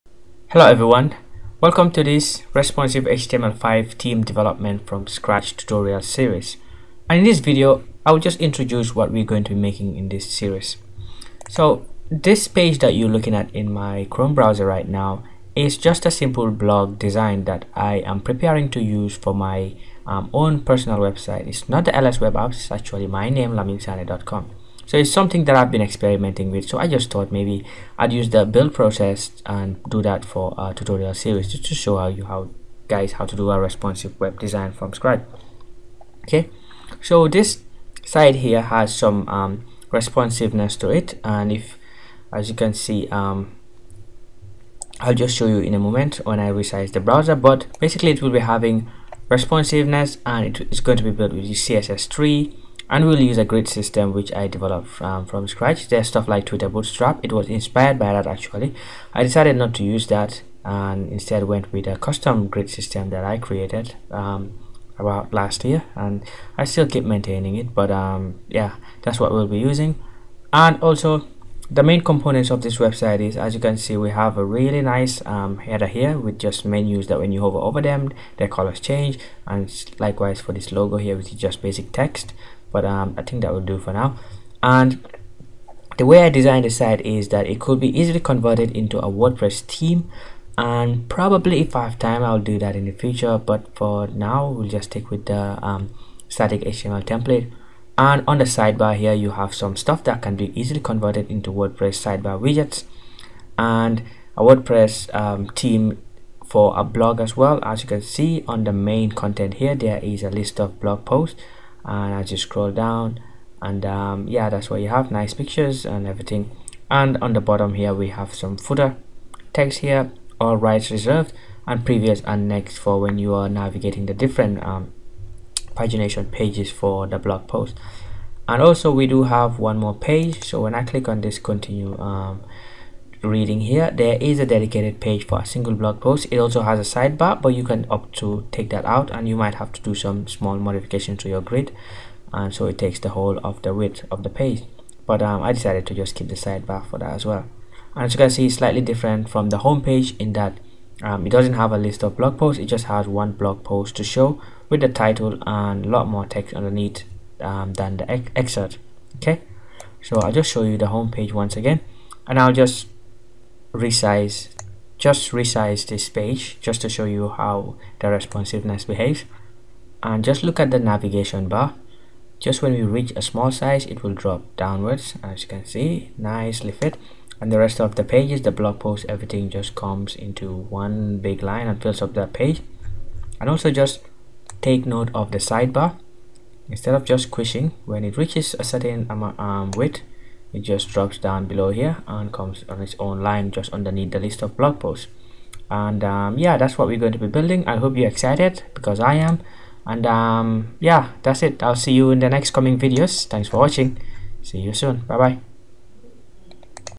Responsive web design with html5 and css3 - Introduction
In this Responsive web design with html5 and css3 tutorial intro video, lets introduce our course. We will highlight the different parts of our theme.